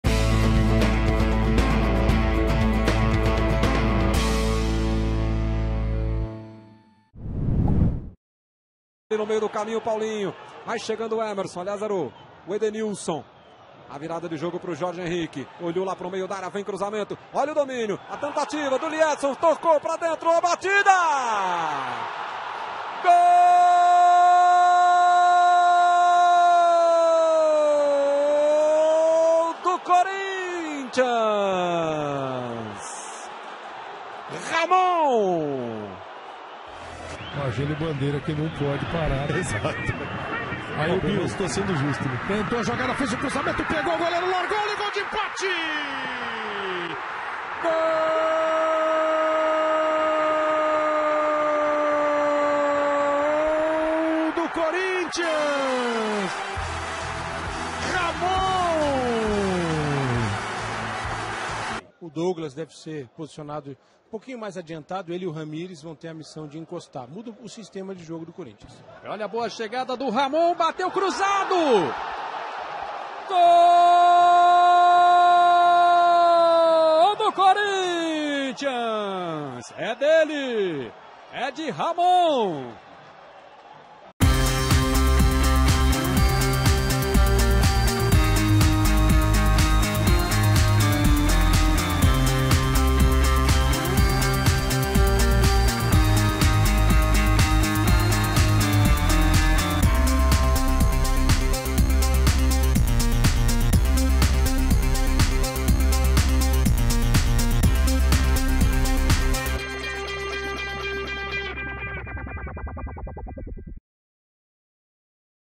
Lateral Ramon! TODOS os gols pelo Corinthians!
Todos os gols do lateral esquerdo Ramon pelo Corinthians.